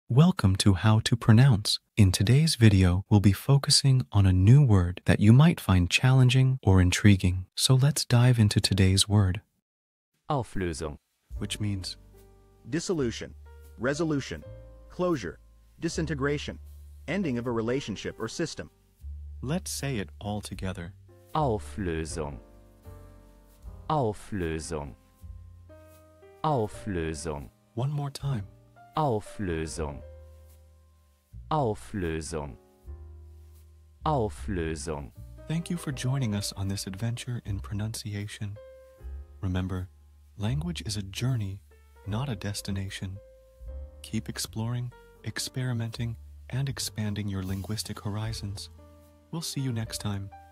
How to pronounce Auflösung
Master the Pronunciation of 'Auflösung which Means resolution' - which means : Dissolution;resolution;Closure;Disintegration;Ending of a Relationship or System. 📝 with @HTPronounce 🎓🗣️ - [Your Guide to German]

Learn to pronounce 'Auflösung' perfectly in German with our easy and engaging tutorial by @HTPronounce! 📖📽️ In this comprehensive video, we provide you with native speaker examples 🗣️👂 and phonetic breakdowns 🎵 to ensure you get the pronunciation just right.

How to say resolution in German